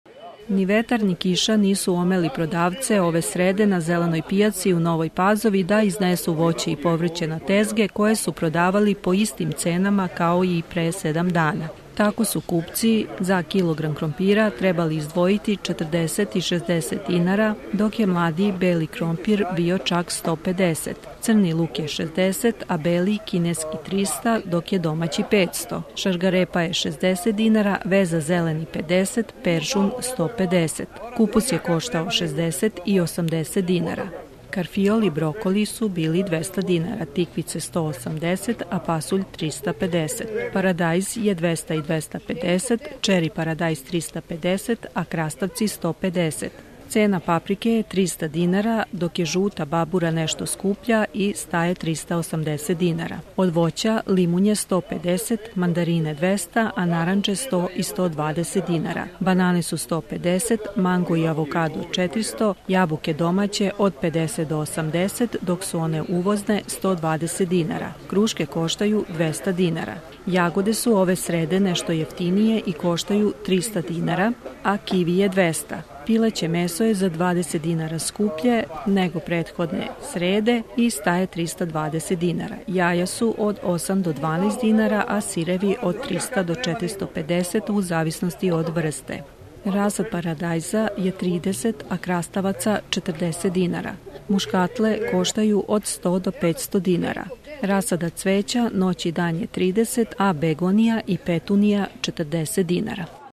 Barometar Nova Pazova
Pojeftinile jagode i koštaju trista dinara a pileće meso poskupelo i sada košta 330 dinara, to je slika jučerašnje pijace u Novoj Pazovi.